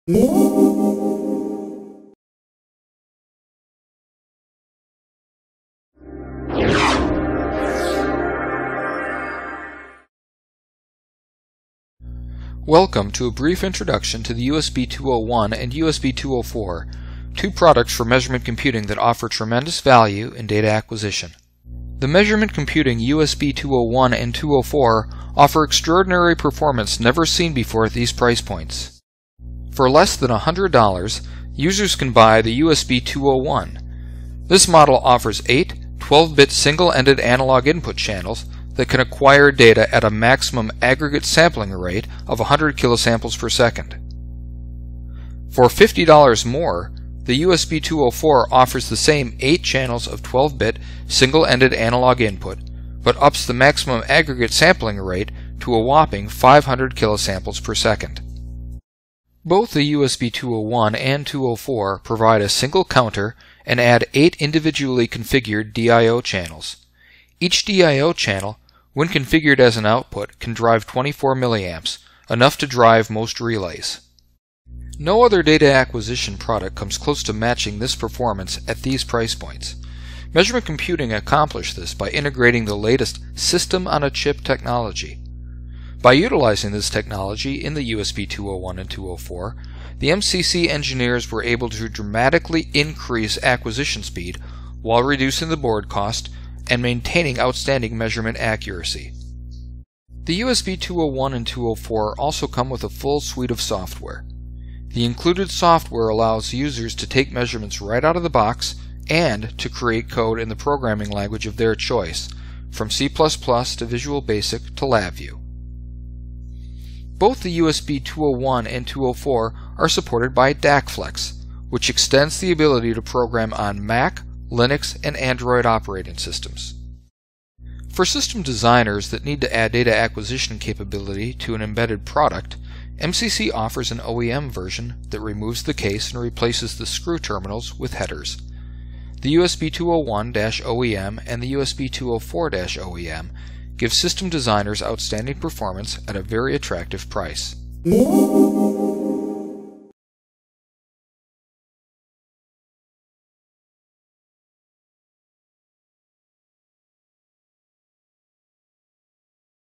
High Speed Low Cost DAQ: introducing the USB-200 series from MCC
Fresh out of Measurement Computing's stable of low-cost USB DAQ devices are the USB-201 and USB-204. These extremely affordable modules pack a lot into their small size yet don't skimp on quality or capability. They deliver 8 single-ended, 12-bit analogue inputs, 8 digital I/O and 1 counter input. The USB-201 has a sample throughput rate of 100 kS/s, while the USB-204 samples at a fast 500 kS/s.

Learn more about the new USB-200 series of DAQ devices from Measurement Computing (MCC) in this short movie.